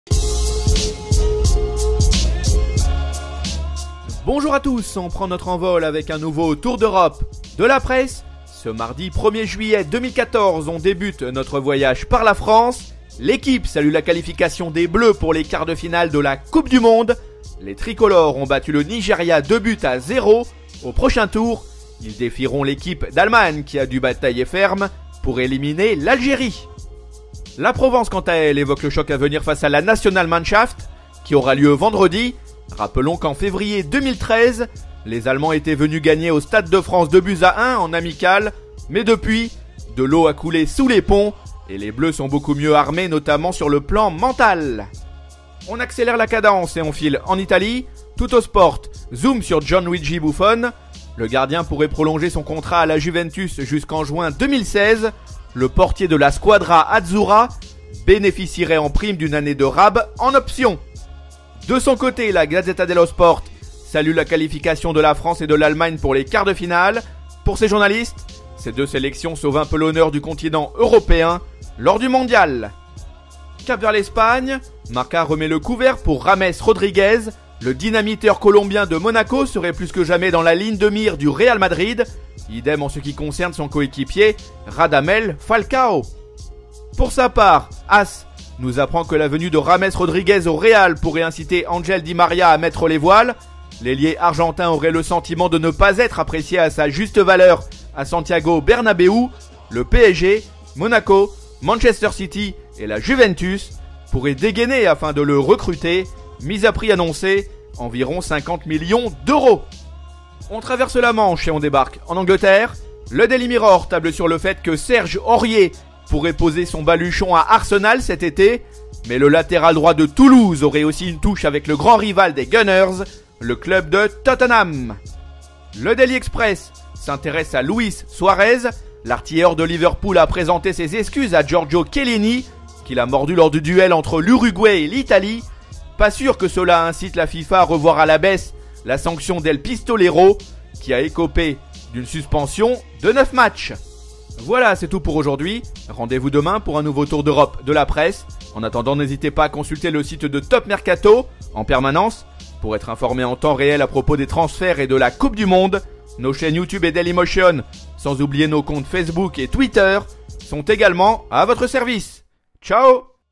La France en quarts, les excuses de Luis Suarez... Le tour de la presse européenne !
Le Tour d'Europe de la presse de ce mardi 1er juillet 2014 est notamment consacré à la qualification de la France pour les quarts de finale de la Coupe du monde. Au programme également, la fièvre James Rodriguez qui s'empare de Madrid ou encore Luis Suarez qui a présenté ses excuses à Giorgio Chiellini.